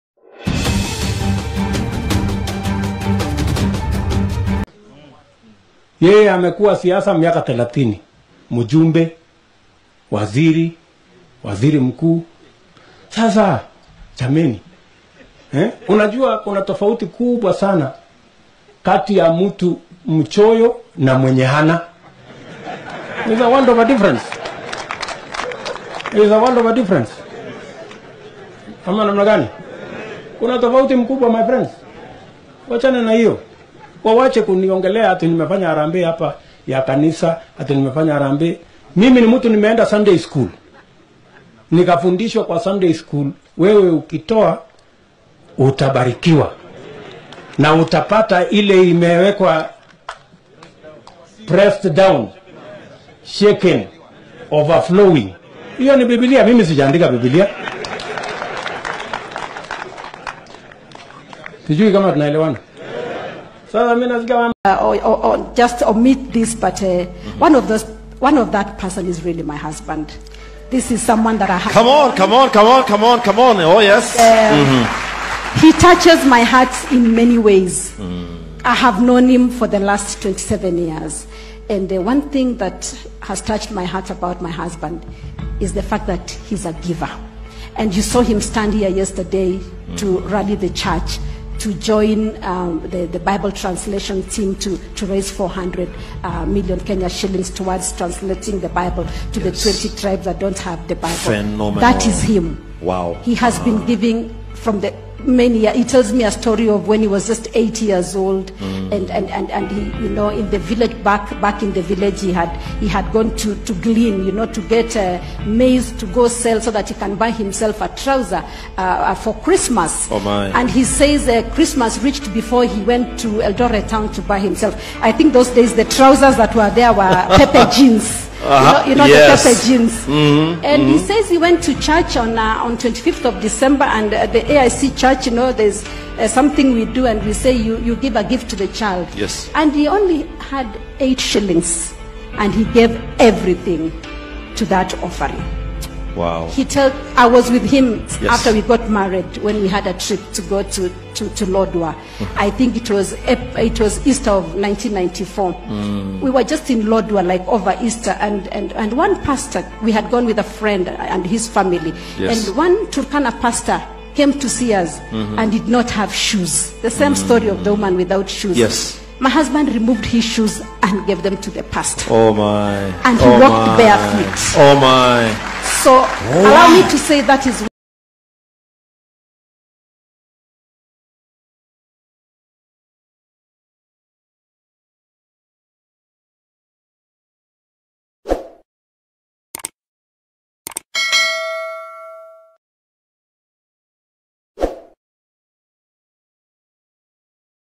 William Ruto embarrasses Raila in public for being stingy🤣🤣🤣🤣🤣🤣🤣🤣
Raila is stingy Ruto says 🤣🤣🤣🤣🤣🤣